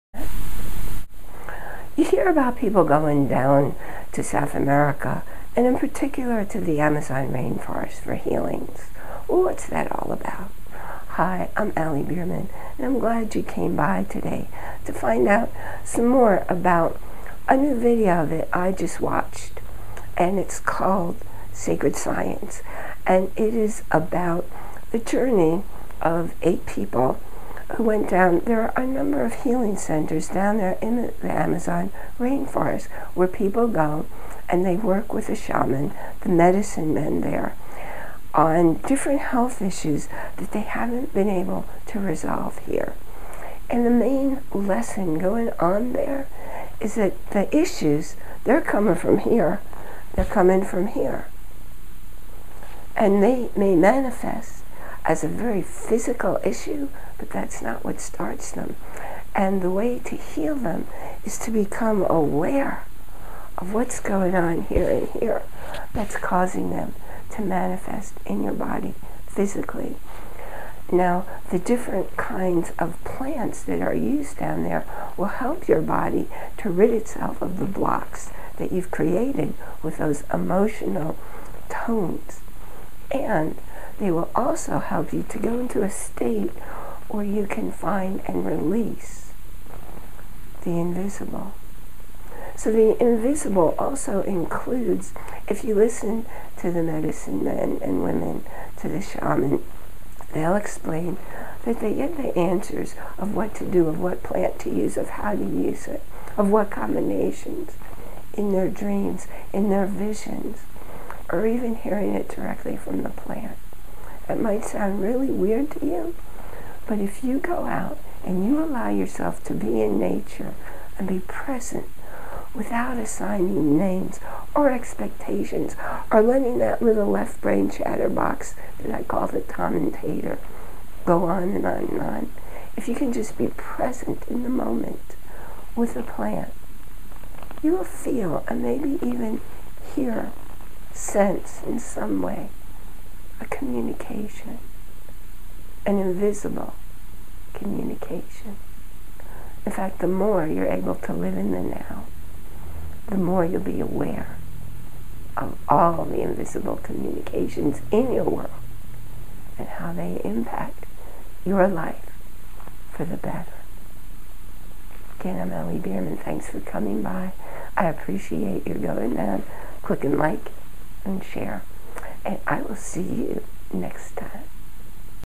Sacred Science and the Amazon Rain Forest
Shaman and medicine men and women know the healing power, passed down for generations, of plants found in the Amazon Rain Forests. they do not work form the thinking mind but rather witness the invisible garnering information form their dreams, visions and listening to the plants themselves.
Be well and happy. In the end, nothing else matters.